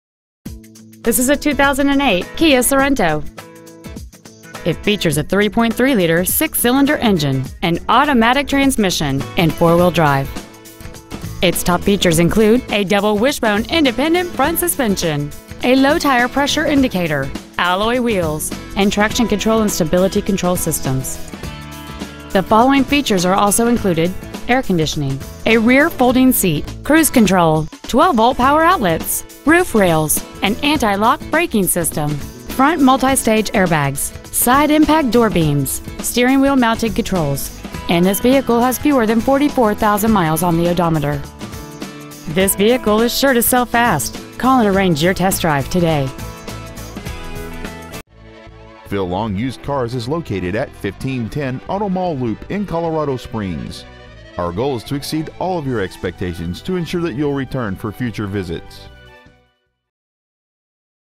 2008 Kia Sorento Denver CO
Year : 2008
 Make : Kia
 Model : Sorento
 Engine : 3.3L DOHC MPFI 24-valve V6 engine
 Trans . : Automatic
 Exterior : Red
 Miles : 43,020
 Interior : Gray

 2008 Kia Sorento Colorado Springs CO